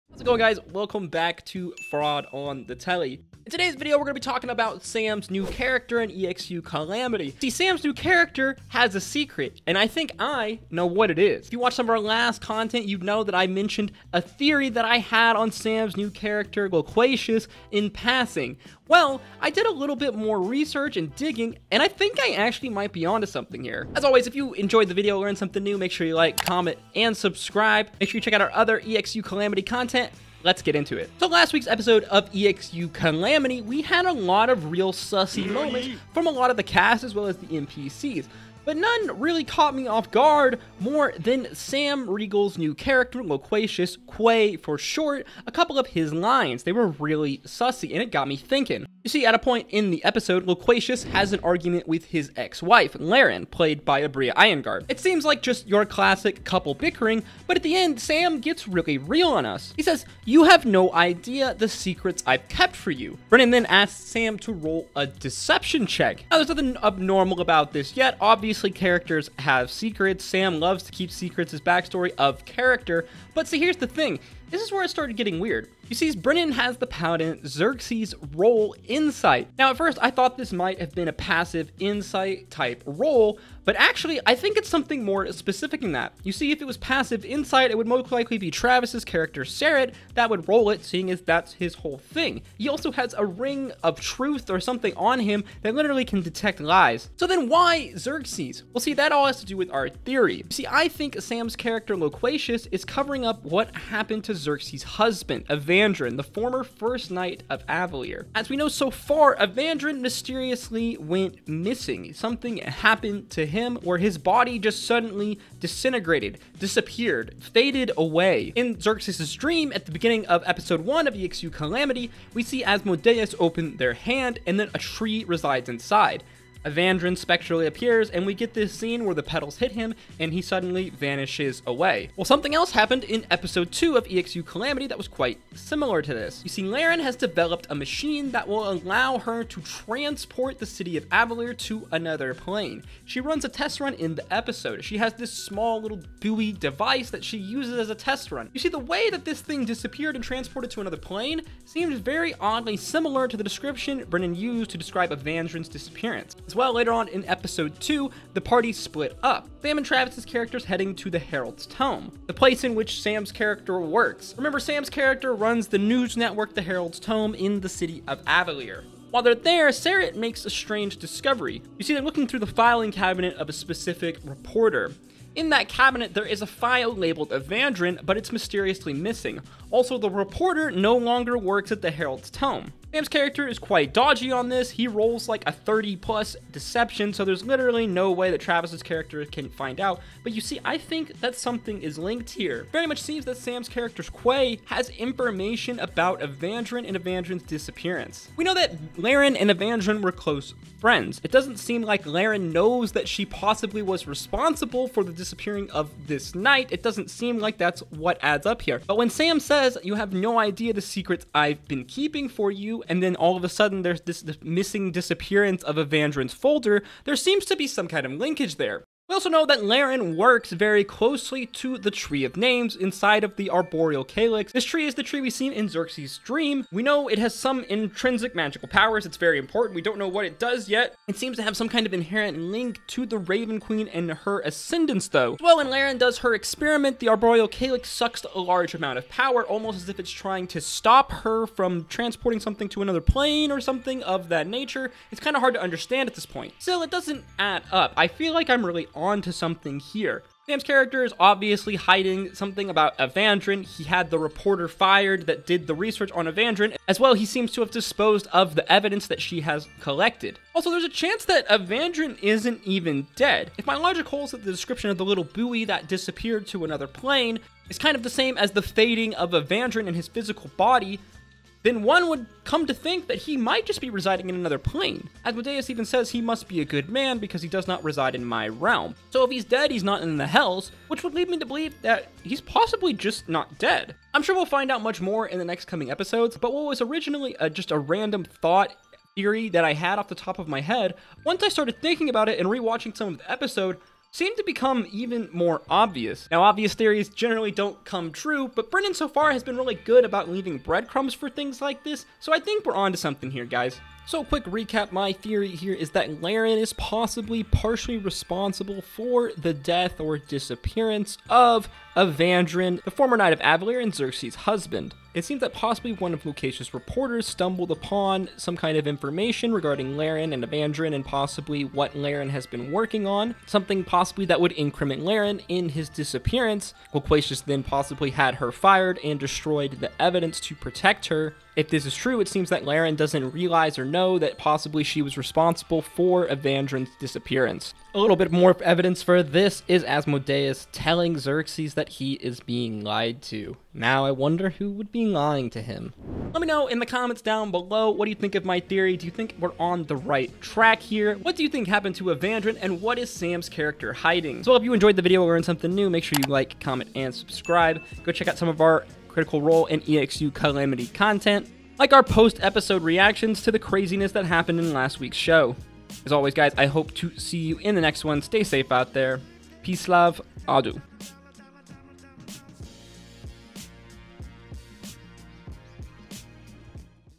Sam's New Character Has A SECRET | Critical Role EXU Calamity THEORY
In this video we discuss Sam's new character in EXU Calamity, and my theory to his massive secret. Sam's new character has a secret and I think I know what it is in this EXU calamity theory.